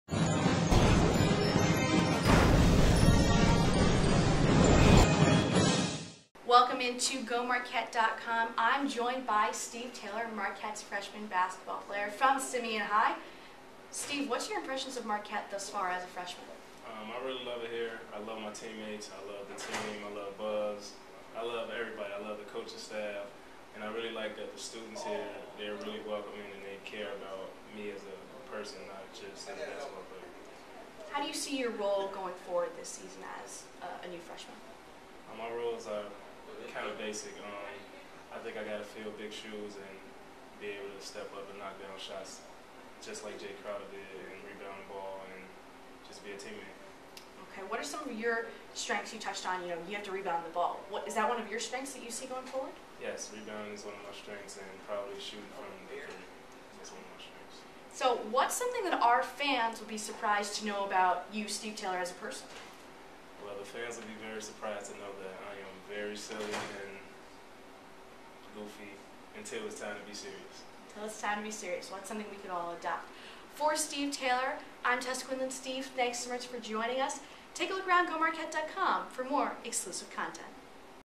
Steve Taylor, Jr. Interview
Steve Taylor, Jr. spoke with GoMarquette.com's Tess Quinlan about what fans can expect from him this upcoming season and his favorite part of being a Marquette student.